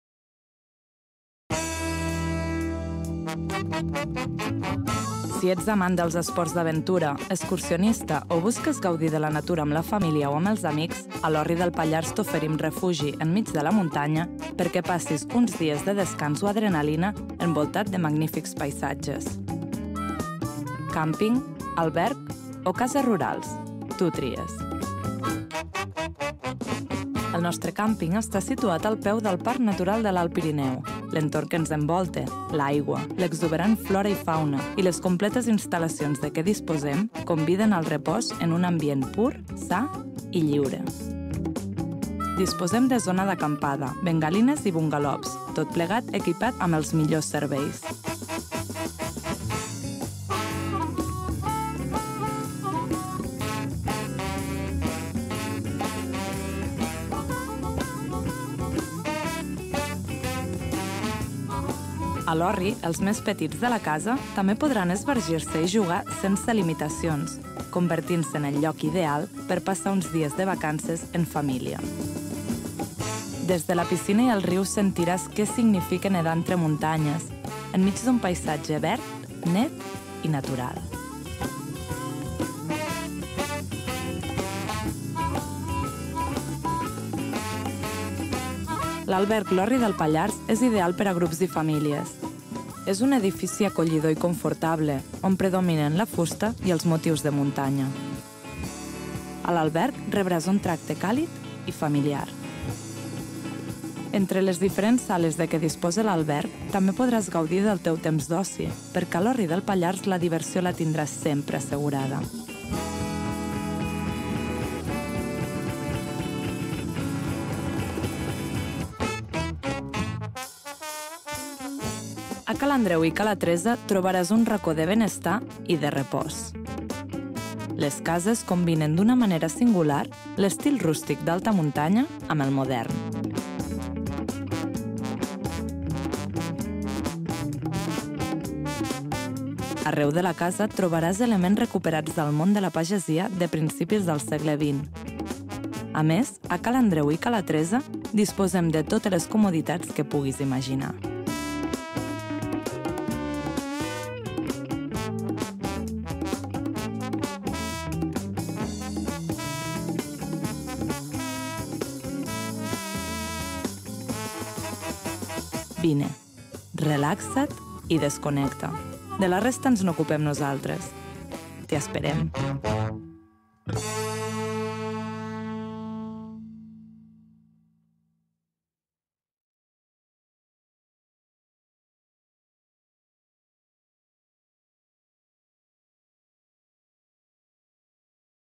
Camping, Albergue y Casas Rurales en Sort. L'Orri del Pallars
Si eres amante de los deportes de aventura, excursionista, explorador o simplemente atrevido...

Si buscas tranquilidad, disfrutar de la naturaleza o comodidad...

Si te animas a escaparte con la familia, quieres divertirte con los amigos o necesitas perderte con la pareja...

En L'Orri del Pallars te ofrecemos refugio en medio de la montaña.

Nos encontramos en el pequeño pueblo de Montardit de Baix, un lugar muy tranquilo ubicado a sólo 3 minutos de Sort, capital del Pallars Sobirà (Lleida). En L'Orri disponemos de tres tipos de establecimientos diferenciados, cada uno adaptado a tus necesidades, donde podrás disfrutar de unos días de descanso o de aventura rodeado de magníficos paisajes.

Camping, albergue o casas rurales, esta es la amplia oferta de alojamiento que ponemos a tu disposición. Tú eliges.